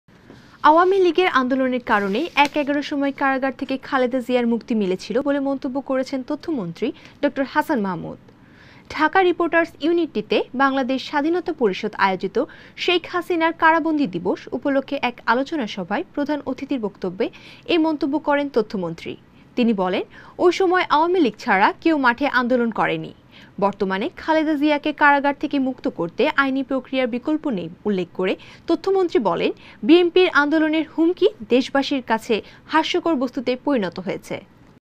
শেখ হাসিনার কারাবন্দি দিবস উপলক্ষে আলোচনা সভা
আওয়ামী লীগের আন্দোলনের কারণেই ১/১১ সময় কারাগার থেকে খালেদা জিয়ার মুক্তি মিলেছিলো বলে মন্তব্য করেছেন তথ্যমন্ত্রী ড. হাছান মাহমুদ।
ঢাকা রিপোর্টার্স ইউনিটিতে বাংলাদেশ স্বাধীনতা পরিষদ আয়োজিত ‘শেখ হাসিনার কারাবন্দি দিবস’ উপলক্ষে এক আলোচনা সভায় প্রধান অতিথির বক্তব্যে এ মন্তব্য করেন তথ্যমন্ত্রী। তিনি বলেন, ওই সময় আওয়ামী লীগ ছাড়া কেউ মাঠে আন্দোলন করেনি। বর্তমানে খালেদা জিয়াকে কারাগার থেকে মুক্ত করতে আইনি প্রক্রিয়ার বিকল্প নেই উল্লেখ করে তথ্যমন্ত্রী বলেন বিএনপির আন্দোলনের হুমকি দেশবাসীর কাছে হাস্যকর বস্তুতে পরিণত হয়েছে।